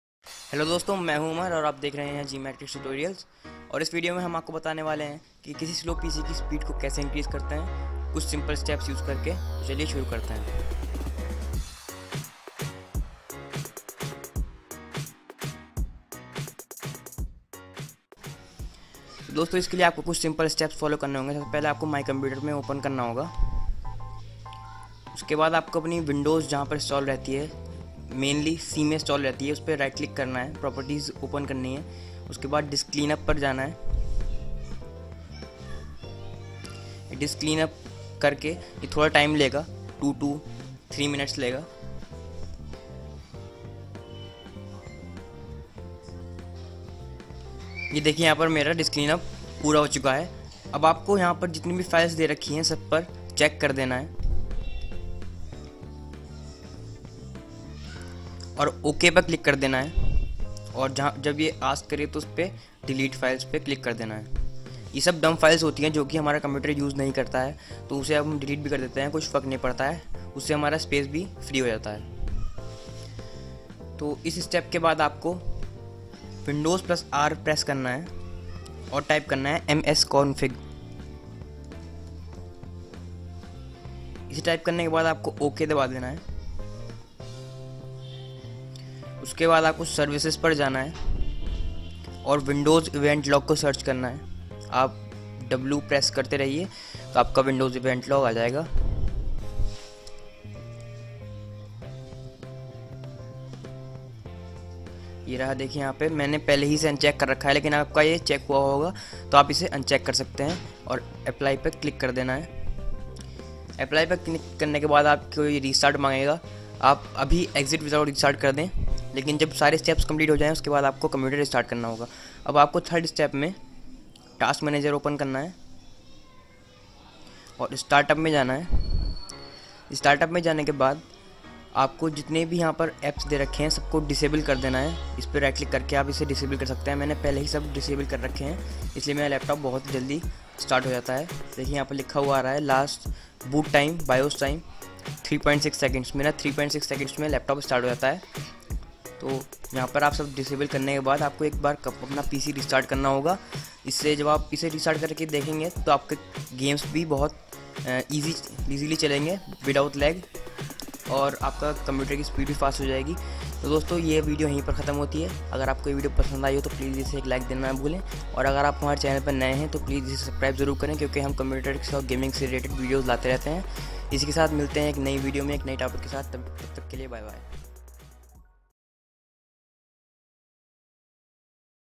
How to speed up a slow pc in just 3 simple steps || 2019
Step 1: disk cleanup
Step 2: peress "win+r" and then "msconfig" and then select services
 disable windows eventlog (also decreases lag or hang in games)
step

note: for windows 7 start up option is available in msconfig only no need to open task manager
step 3: open task manager and goto to startup menu and disable every app

you are done just restart your pc and feel the change